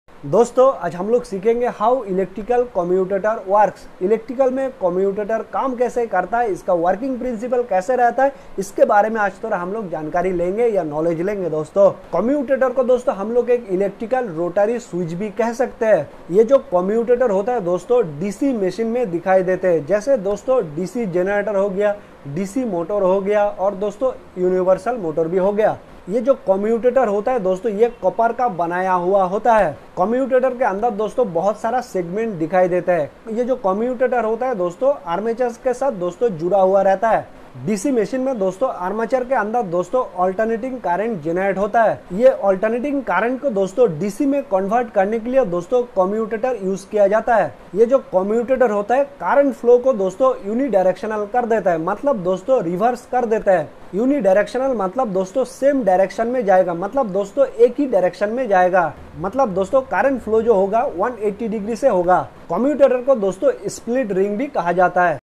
How Electrical Commutator Works ? || Electrical Commutator Kaam Kaise Karta Hai ?
How Electrical Commutator Works ? || Electrical Commutator Kaam Kaise Karta Hai ? || This video briefing about how electrical Commutator Works...